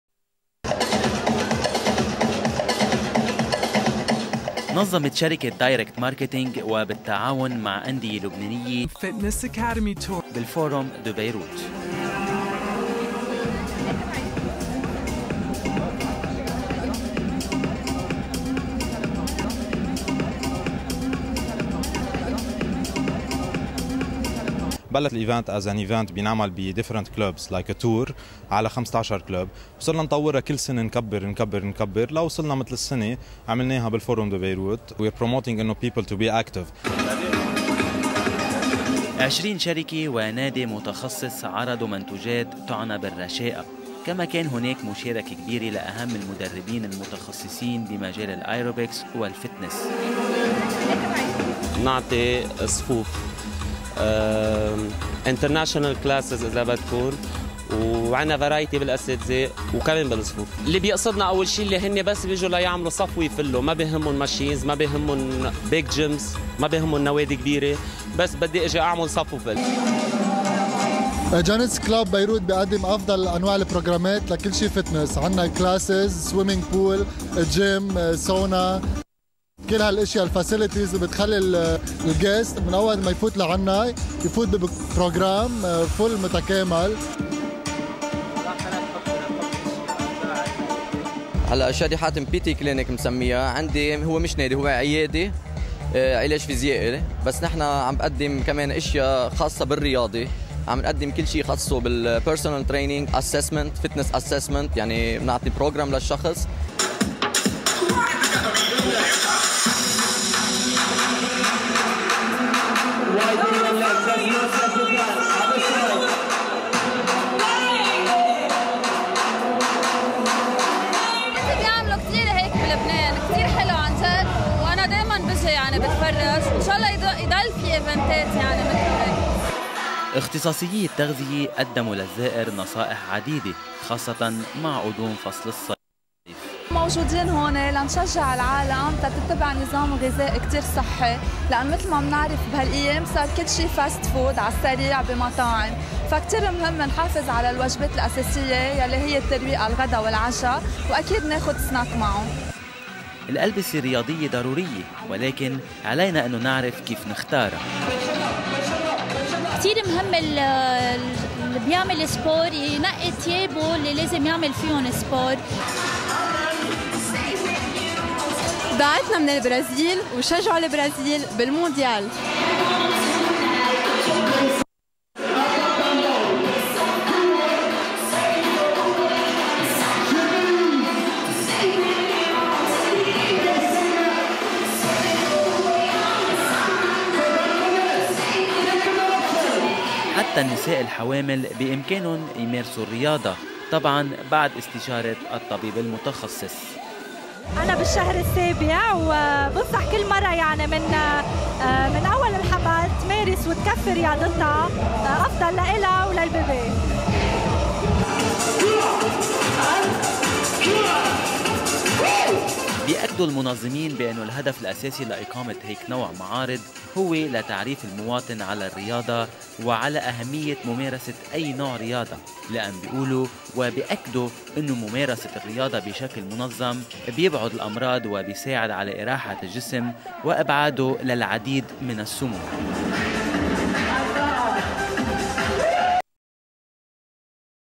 Orbit TV Report on the 2010 Fitness Academy Tour
Orbit TV featured a report on the 2010 Fitness Academy Tour closing event at the Forum de Beyrouth.

We knew that year after year, the buzz around the Fitness Academy Tour was growing. But we didn't know how popular the event had become until our fifth anniversary.

Held at Forum de Beyrouth, the 2010 Fitness Academy Tour was the biggest edition to date with more people joining the event and new workouts to challenge them.

Whim Gym and Genesis Club made their first appearance at the event, offering a variety of group fitness training programs from muscle building exercises, freestyle step and boot camp workouts to high-low aerobics and Zumba classes, which were introduced for the very first time. The event was exhilarating for both the participants and us.